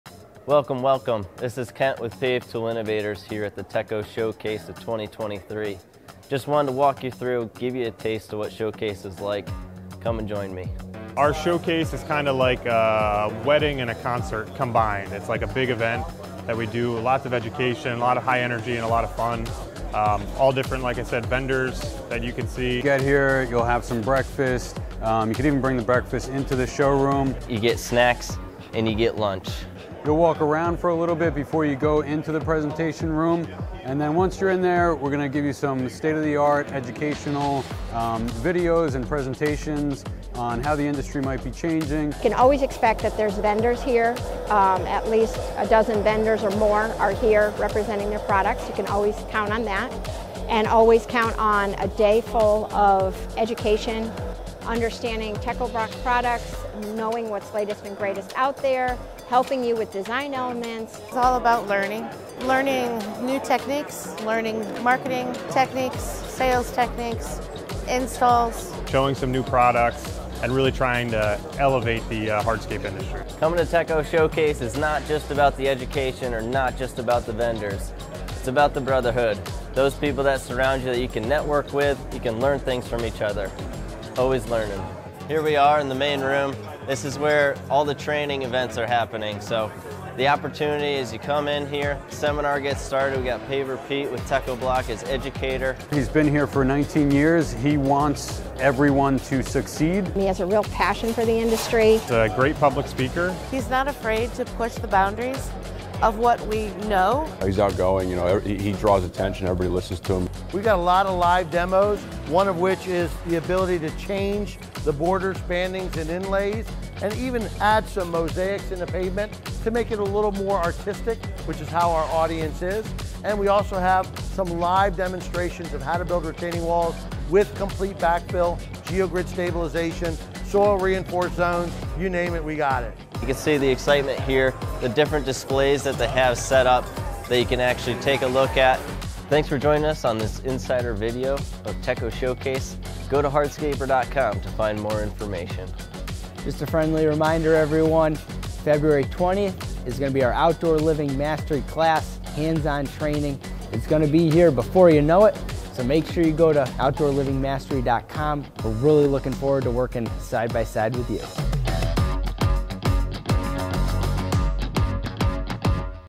Techo-Bloc Show Experience
Join us in the immersive excitement of a @techobloc
@hardscaper educational event from an inside perspective.